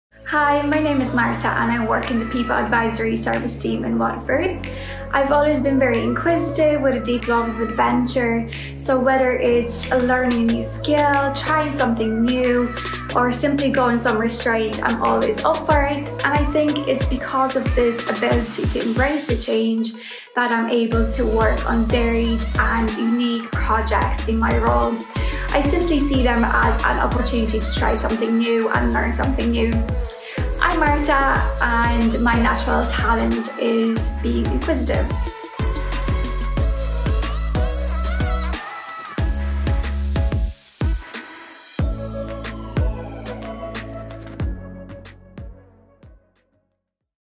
Marta - EY Graduate Programme 2021
Everybody has a natural talent. At EY, we want to help you develop yours, naturally.
Say hi to Marta! Her natural talent is being naturally inquisitive. Find out how her love of adventure helps her in our Graduate Programme at EY!

Apply to the EY Graduate Programme 2021 here: ey.com/en_ie/careers/what-you-can-do-here/student-programmes